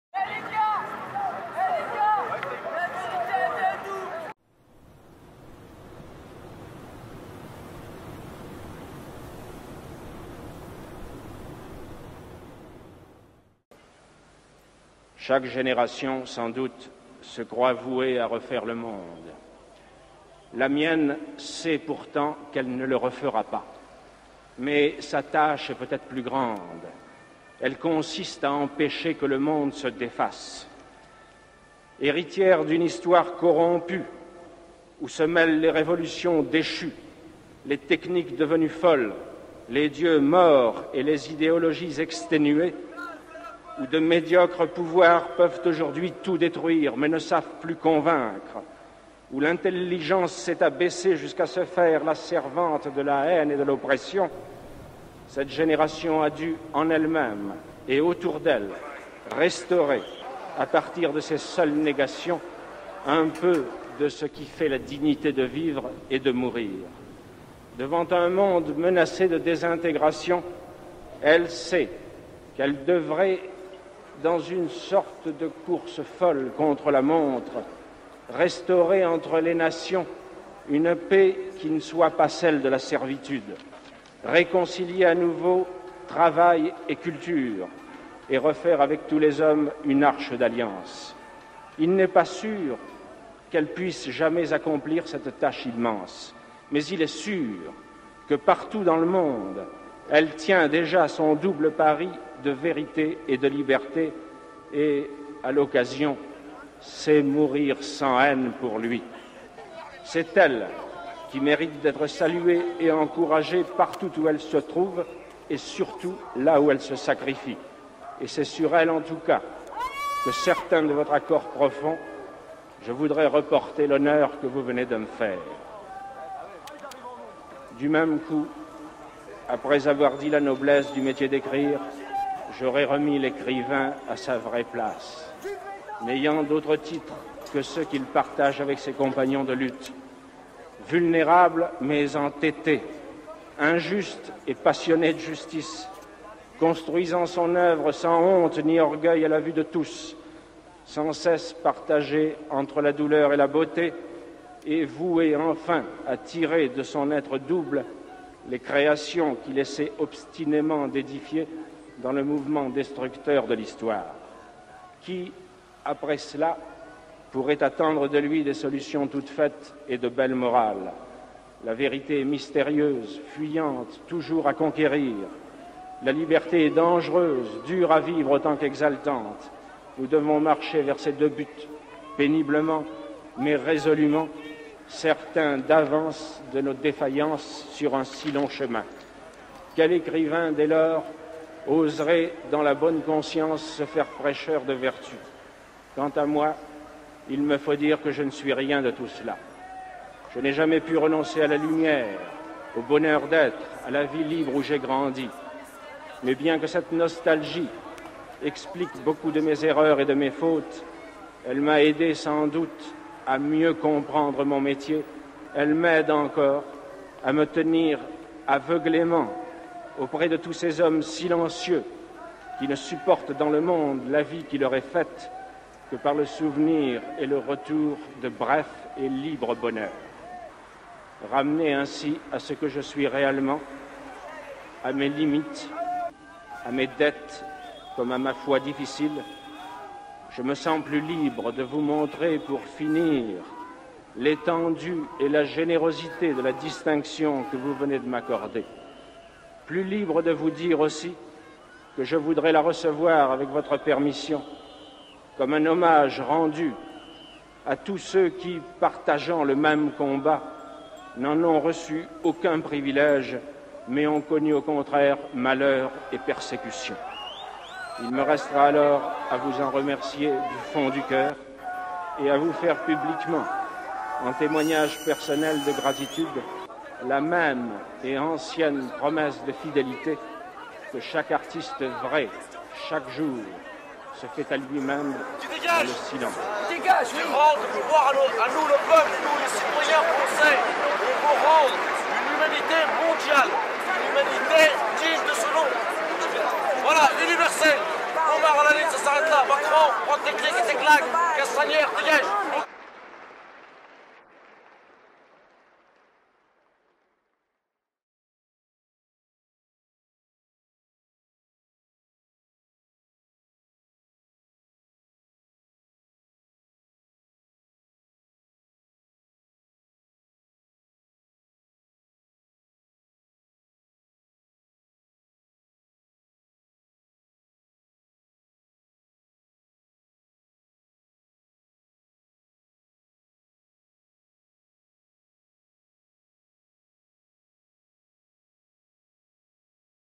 La vérité est à nous - 19 février 2019 - Paris - Gilets jaunes
Compte-rendu vidéo de la soirée du dimanche 17 février 2019 - Place de la République, Bastille et boulevard Voltaire. Fond sonore : discours d'Albert Camus lors de sa remise du prix Nobel de littérature en 1957 (Stockholm). Inspirant et toujours d'actualité.